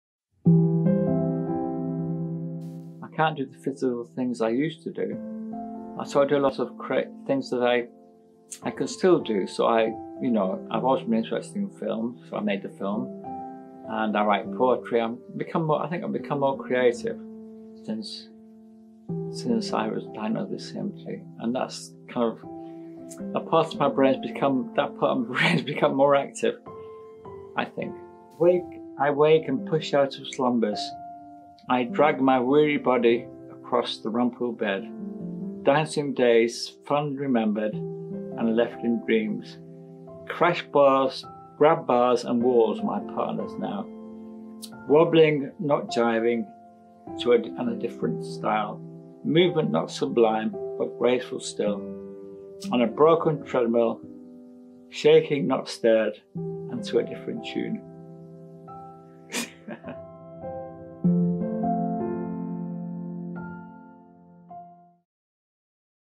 Living with CMT
Douglas, participant in the CMT&Me study shares what it means to him to live with Charcot-Marie-Tooth disease.